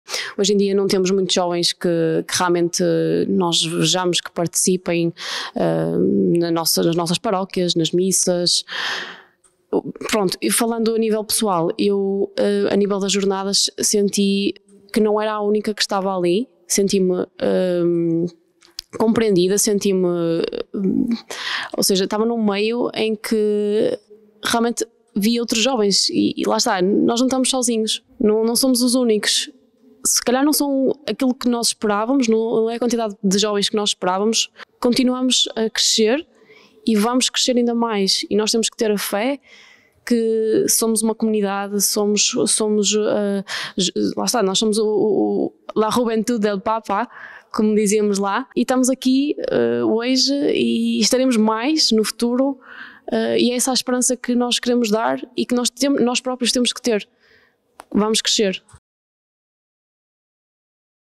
"Não estamos sozinhos" - Daniela Gomes
O que esperar da Igreja e dos jovens? Passado um ano da JMJ Lisboa 2023, o que pensam alguns jovens da Arquidiocese de Braga sobre o futuro.